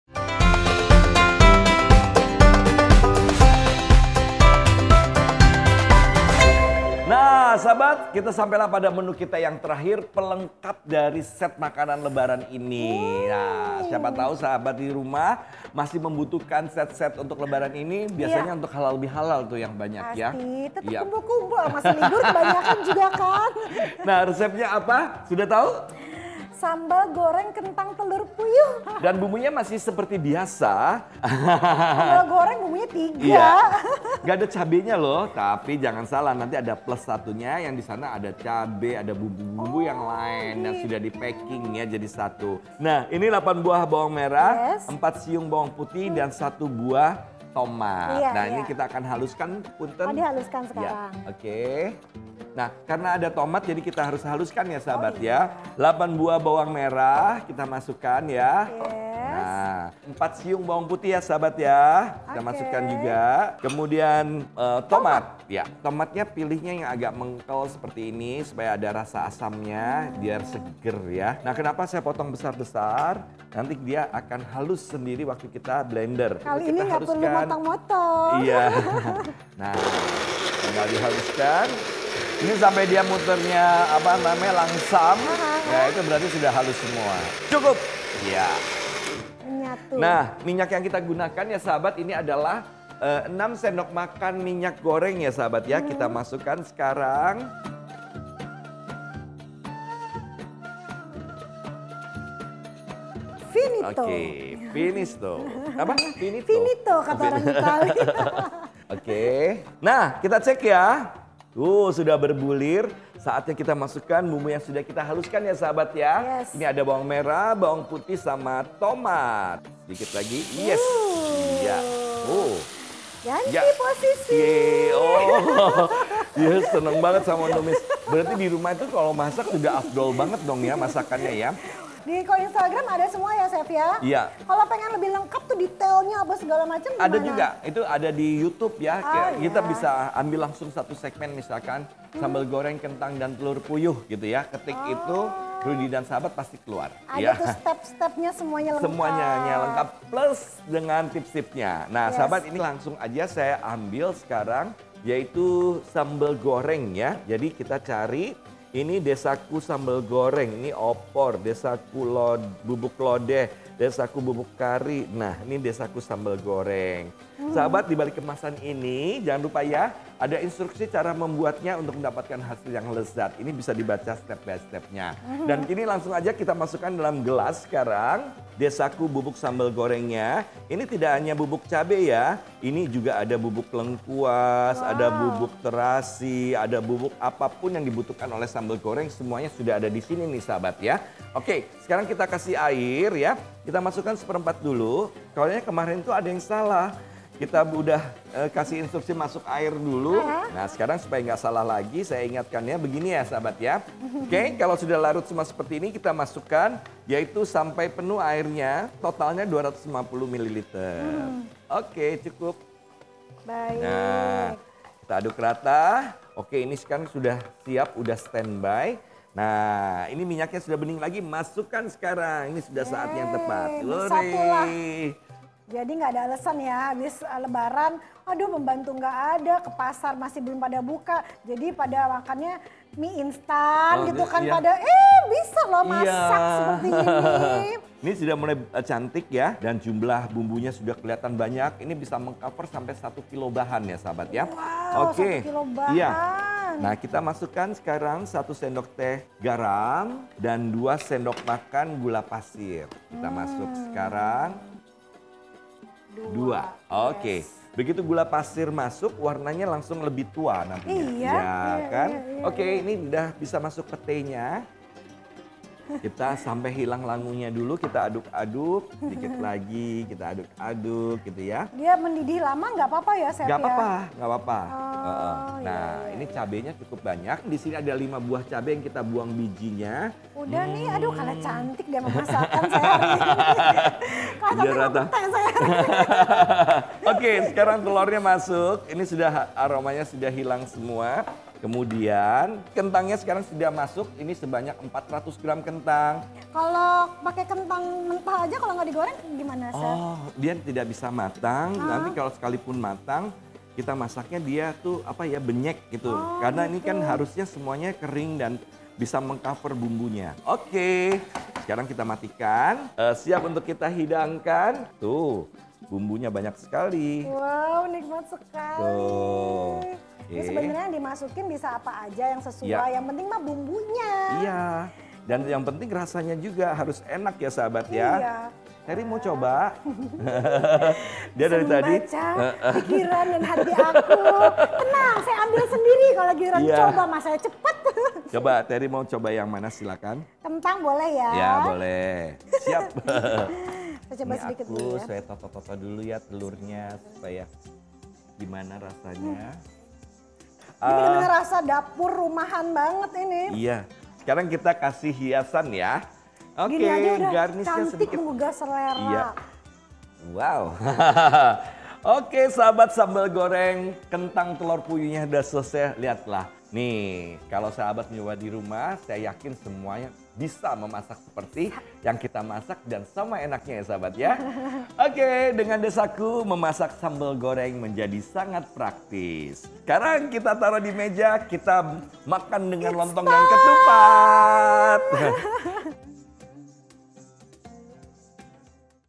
RUDY & SAHABAT - SAMBAL GORENG KENTANG & TELUR PUYUH
Bahan 1:
25 butir telur puyuh direbus dan dikupas
400 g kentang potong dadu dan digoreng
15 petai dibelah 2 bagian
5 buah cabe merah, dibelah, buang biji, rajang halus

Bahan 2: (dihaluskan)
8 buah bawang merah
4 siung bawang putih
1 buah tomat

Bahan 3:
6 sdm minyak goreng
250 ml/ 1 gelas air
1 saset Desaku Bubuk Sambal Goreng
1 sdt garam
2 sdm gula pasir

Cara Membuat:
• Haluskan bahan 2
• Kemudian tumis dengan 6 sdm minyak goreng sampai harum
        dan matang
• Larutkan Desaku Bubuk Sambal Goreng dengan satu gelas air,
        aduk rata lalu masukkan ke dalam tumisan yang sudah tanak,
        masak kembali hingga mendidih dan air mulai berkurang
• Masukkan  garam dan gula pasir, aduk hingga garam dan gula
        pasir larut
• Jika sudah mulai mengental bumbunya, masukkan cabe merah
        dan petai, tumis kembali hingga aroma langu cabe dan petai
        hilang
• Masukkan telur puyuh dan kentang aduk rata hingga semua
        bahan terbalut dengan bumbu
• Angkat dan sajikan